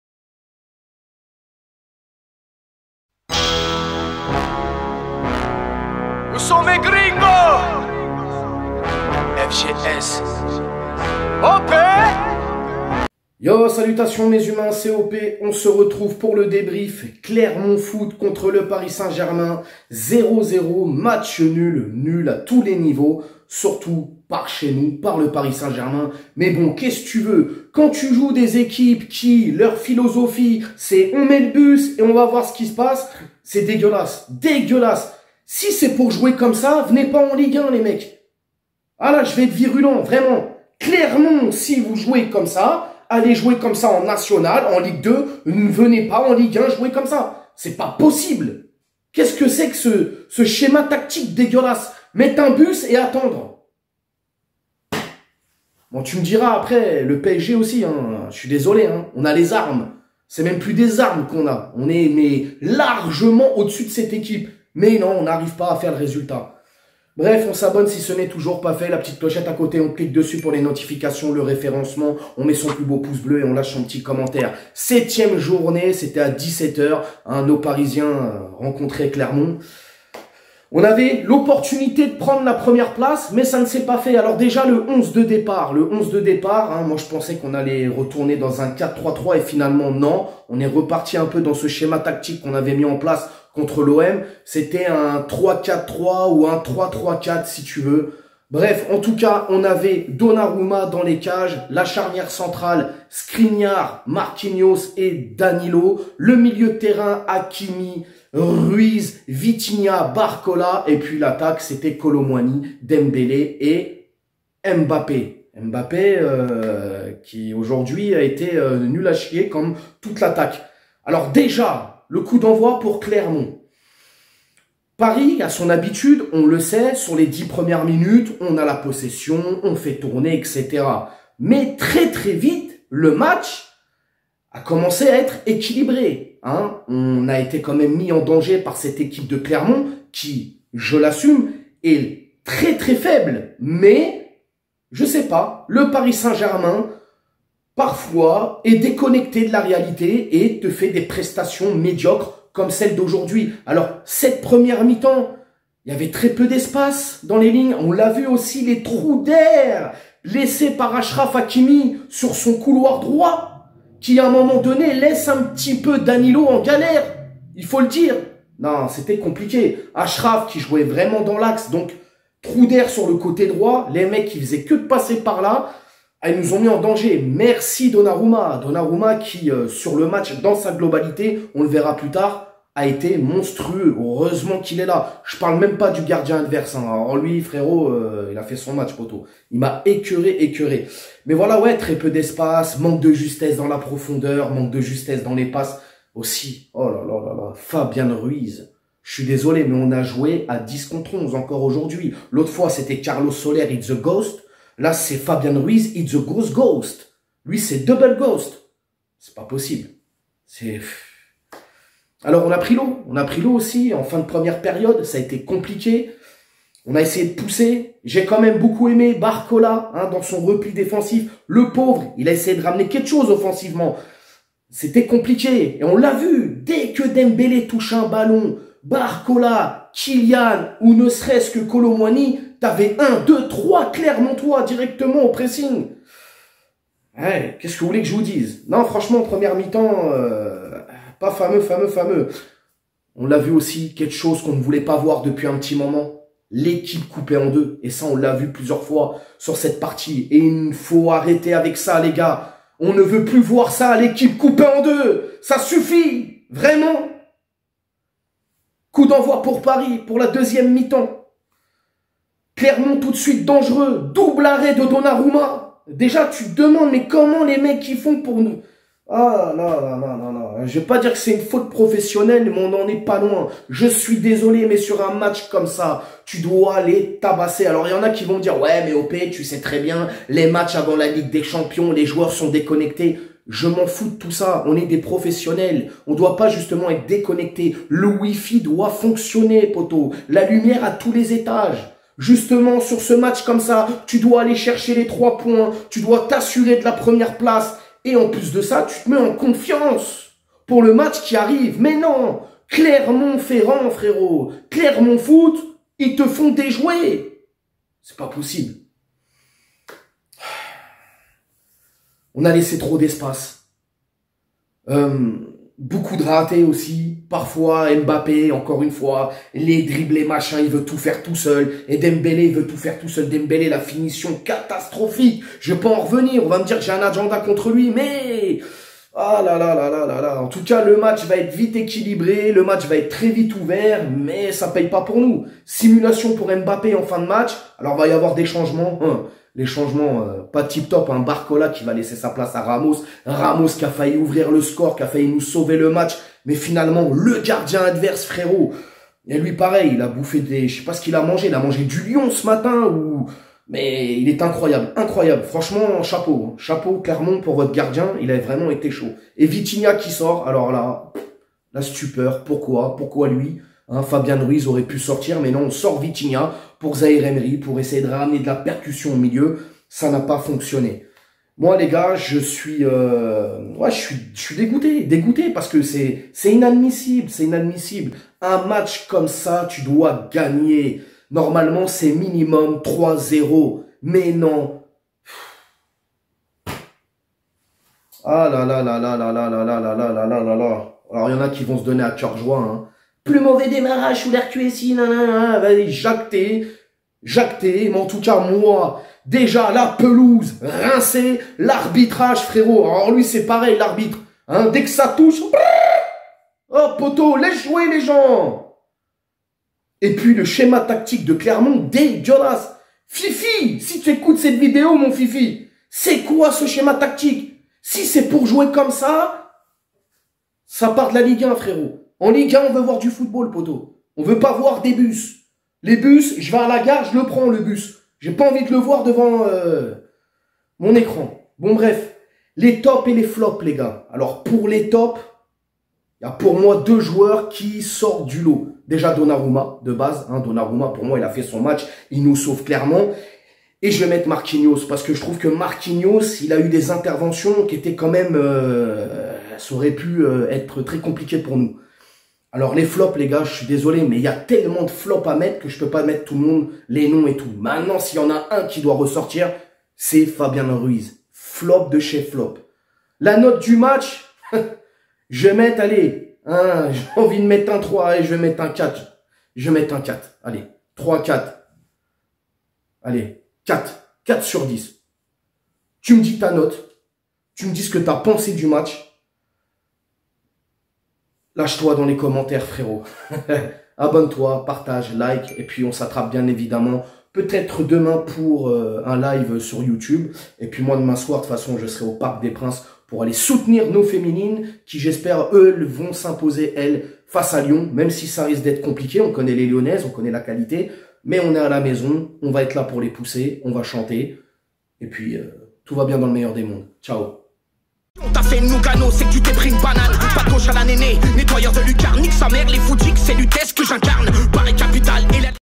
🤬C'EST QUOI ÇA ? LA HONTE ! (CF63 0-0 PSG)
Le PSG s'est littéralement loupé face à Clermont avec un pitoyable "0-0" pour cette 7ème journée de Ligue 1, une équipe parfois coupé en deux et un manque de finition 😩 on en discute dans cette vidéo 🤝